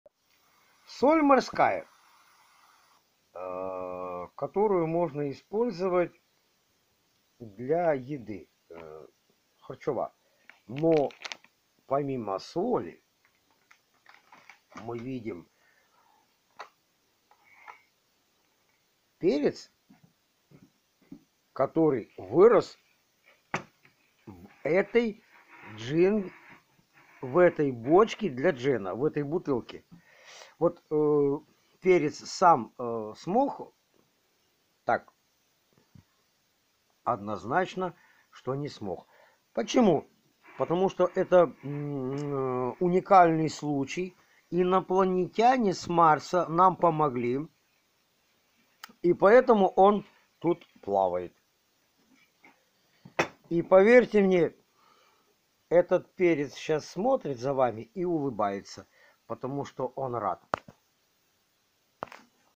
Водоплавающий перец🌶️ 1L1691
Водоплавающий перец🌶️. морская которую можно использовать для еды харчева Но помимо соли мы  видим перец который вырос в этой джин в этой бочке для Джена в этой бутылке вот перец сам смог так однозначно что не смог Почему Потому что это уникальный случай инопланетяне с Марса нам помогли и поэтому он тут плавает и Поверьте мне этот ц сечас смотрит за вами и улыбается он рад,